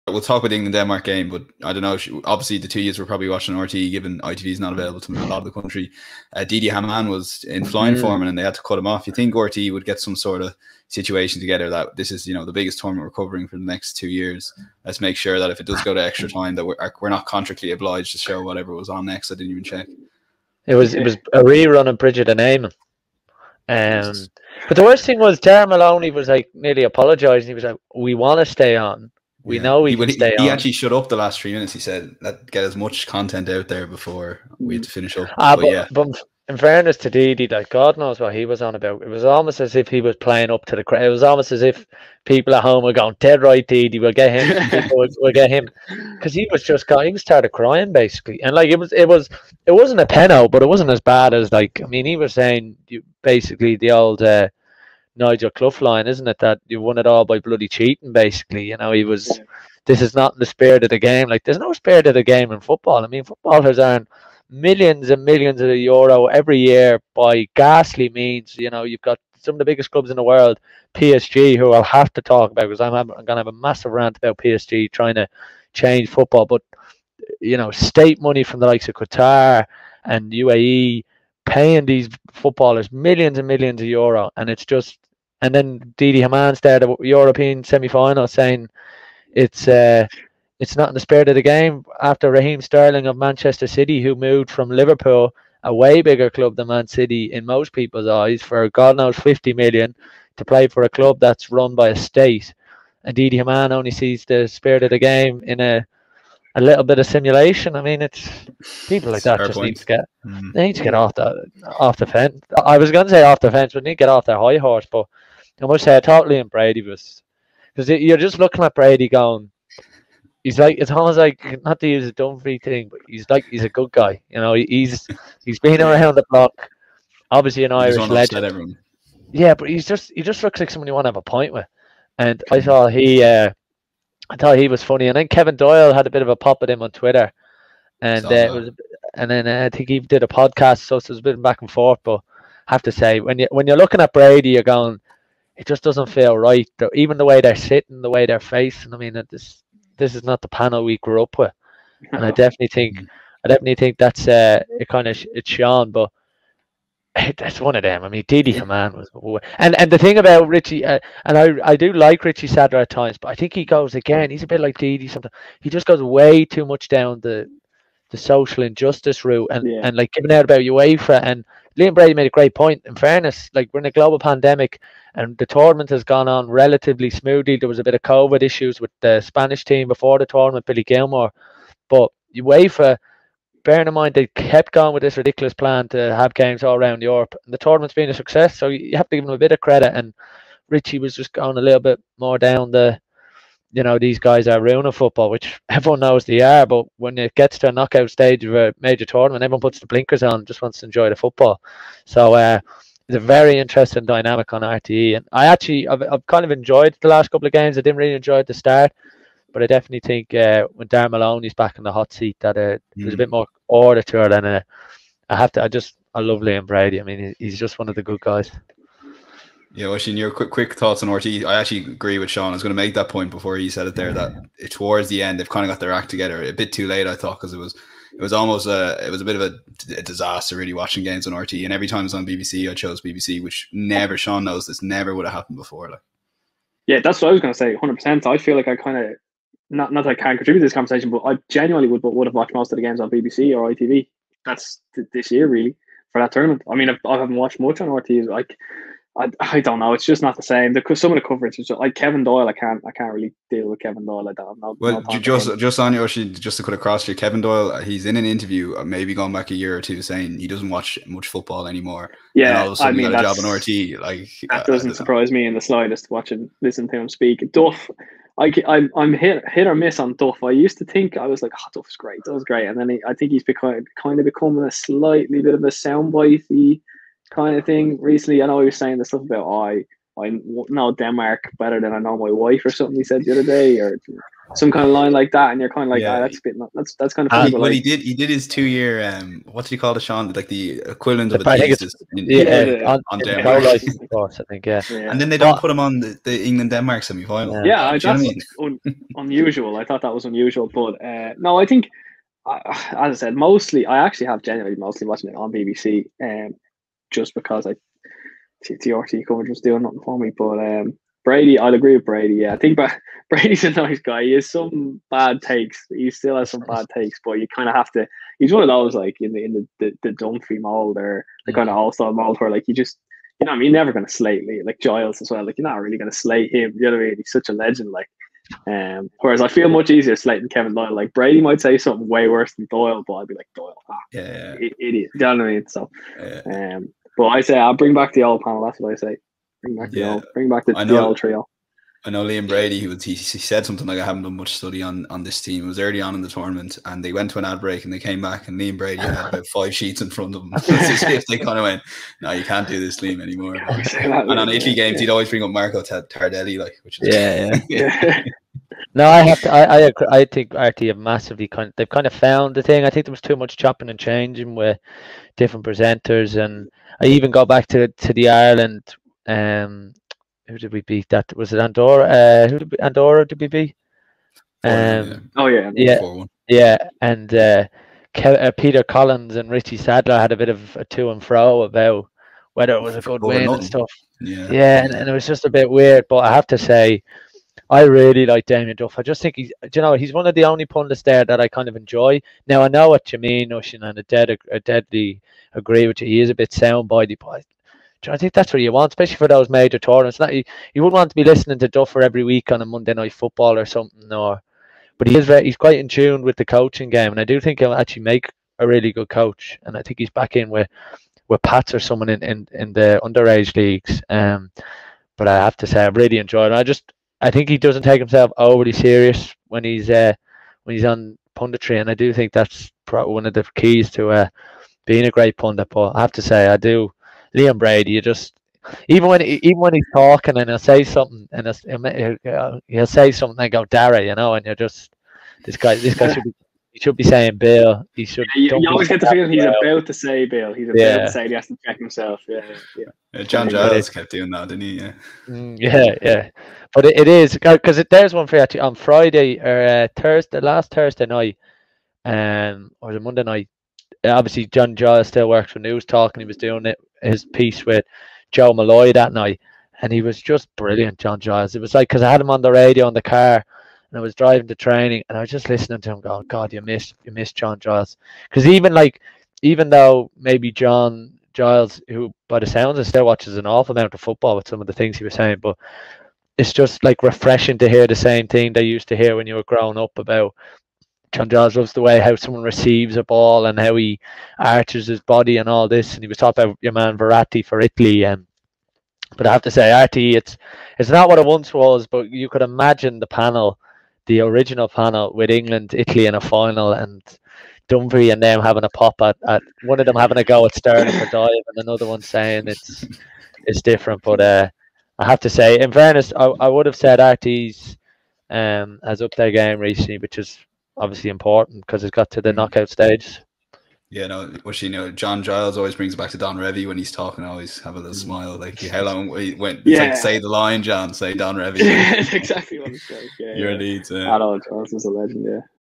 The RTE Euro 2020 Panel Review - Richie Sadlier, Liam Brady and Didi Hamann
We miss the old RTE panel of John Giles, Eamon Dunphy, Liam Brady and the late Bill O'Herlihy.

So how does the Euro 2020 team compare? Well they have finally found their feet late in the tournament with Richie, Liam and Didi forming a formbidable trio on Wednesday night with plenty of debate.